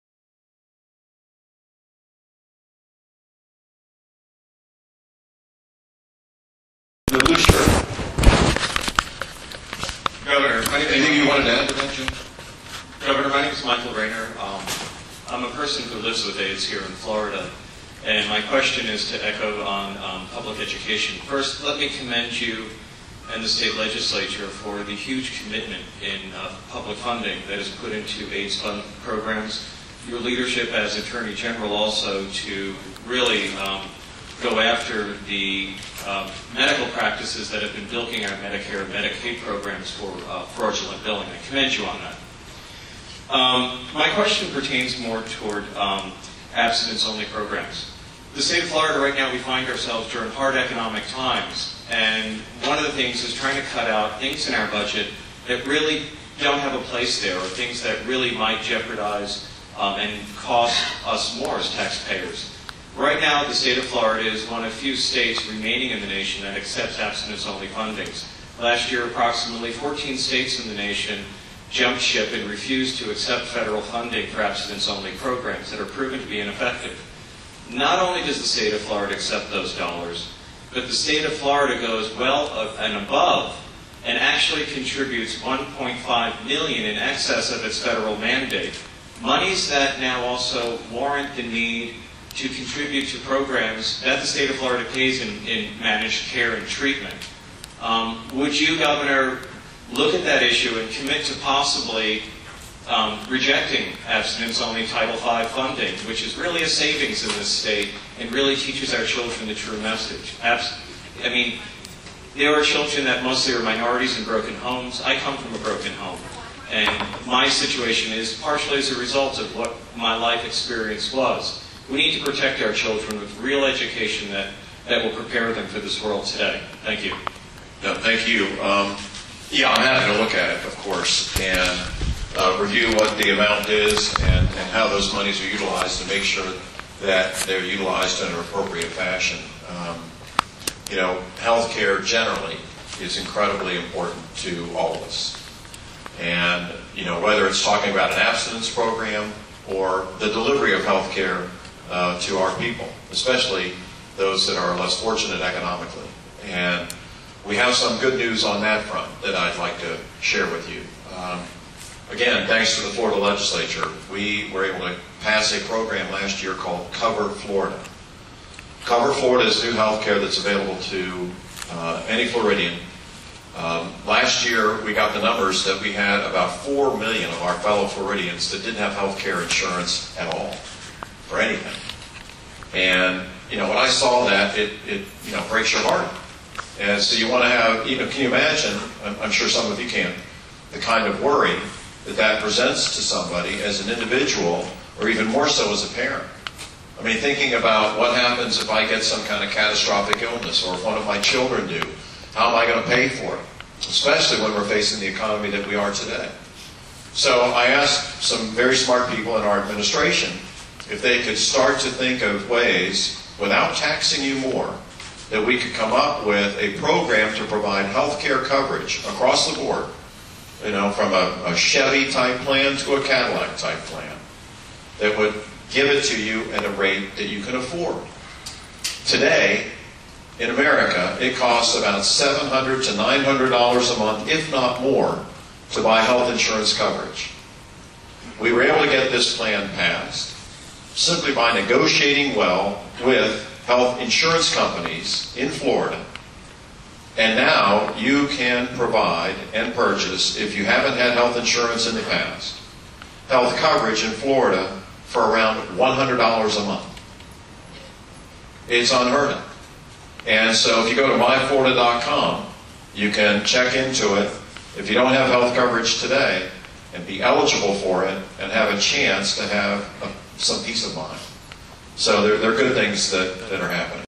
Charlie Crist to Review Title V Funding
At a town hall meeting in Ft. Lauderdale on February 18, 2009, Florida Governor Charlie Crist was asked a question about government inefficiencies and wasting taxpayer dollars on ineffective abstinence-only programs.

In response, Governor Crist said that he will "review abstinence-only funding."